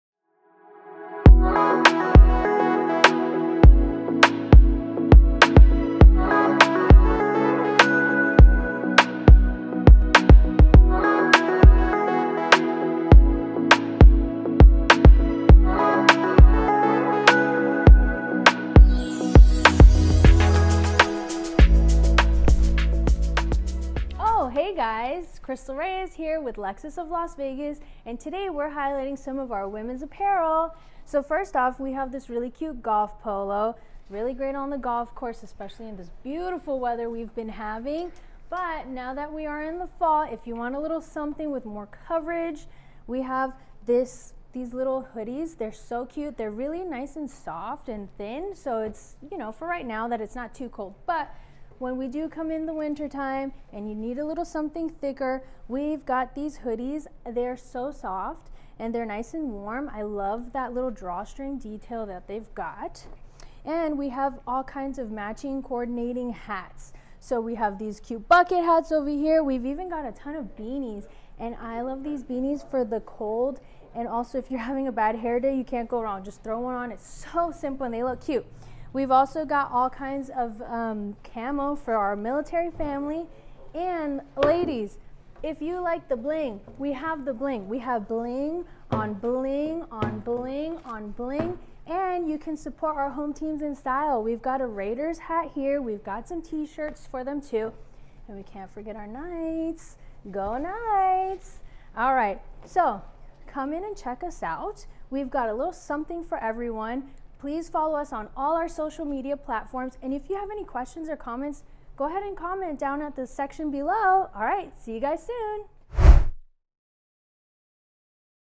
HATS HOODIES AND SHIRTS- Lexus of Las Vegas- Lexus Genuine Accessories
HATS HOODIES AND SHIRTS ….. Krystal Reyes show us the selection of Women’s Lexus merchandise available at Lexus of Las Vegas.